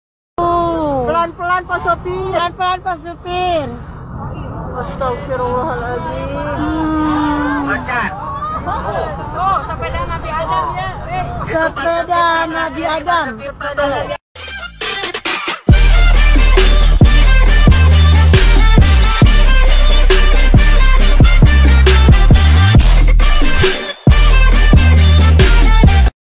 seorang emak-emak nyetir pick up di malam hari sambil ngebut. !
Viral seorang emak-emak nyetir pick up di malam hari sambil ngebut. !

Lokasi : Turikale, Maros, Sulsel.